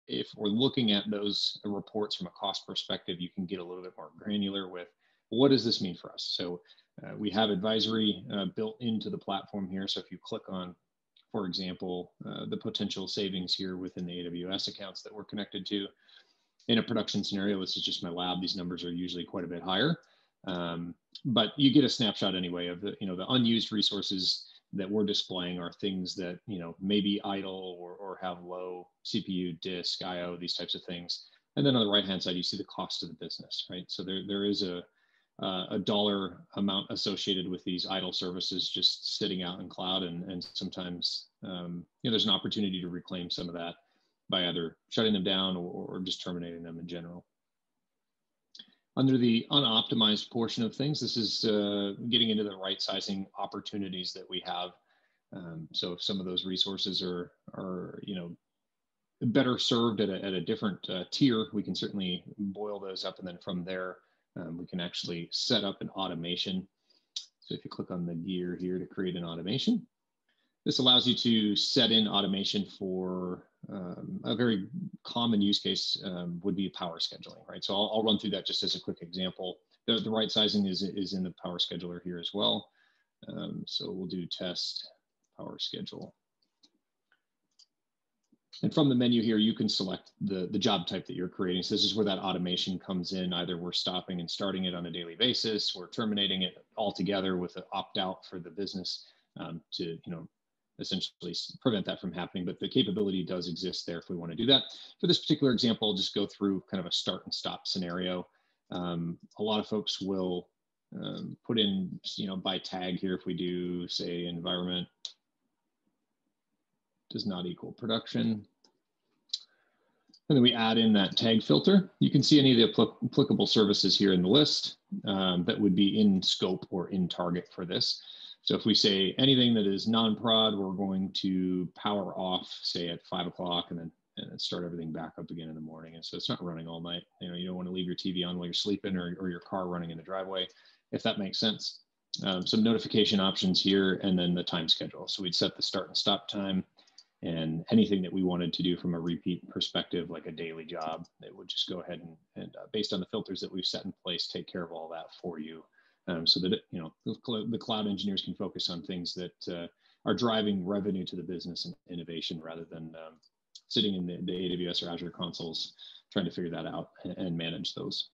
Optimizing cost using CloudBolt's Cost and Security Optimization Service Adviser
In this brief explainer video, learn how to deep dive into your savings with the service adviser from Solutions Engineer, Josh Mau.